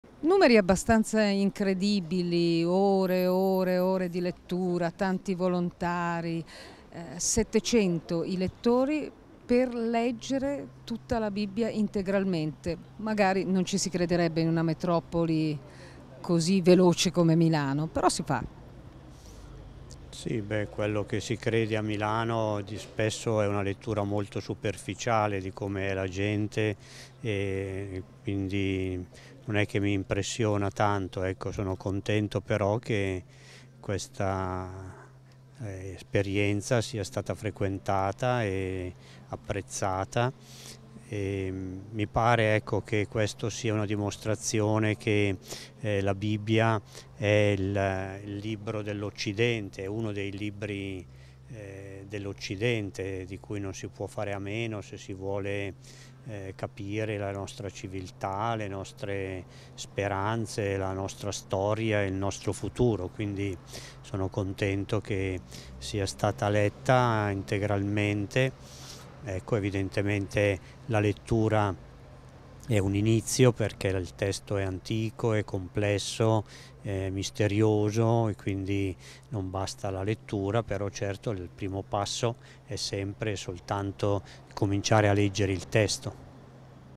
“Incontriamo la Parola”: lettura della Bibbia - intervista a mons. Mario Delpini
Delpini e Sala hanno chiuso “Incontriamo la Parola”
Questo pomeriggio, alla cappella San Giorgio della Casa Scout di via Burigozzo 11 (Milano), con gli interventi dell’Arcivescovo, monsignor Mario Delpini, e del Sindaco Giuseppe Sala, si è conclusa “Incontriamo la Parola”, la lettura integrale e ininterrotta della Bibbia promossa da Ente e Fondazione Baden.

Gli interventi dell’Arcivescovo e del Sindaco al termine dell’originale maratona biblica promossa da Ente e Fondazione Baden.